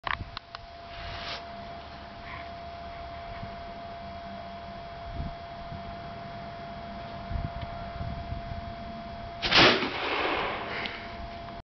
1 inch bore Hakenbuch at range
100 caliber handgone 16  .395 balls and 100 gr 2F powder. I need better match.